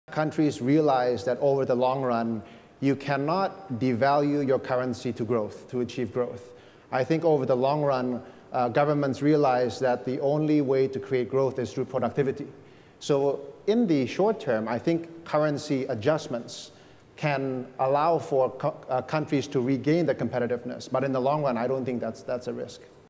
In Markets We Trust - John Riady
“You cannot devalue your currency to achieve growth. The only way to create growth is through productivity.” John Riady, Executive Director, Lippo Group.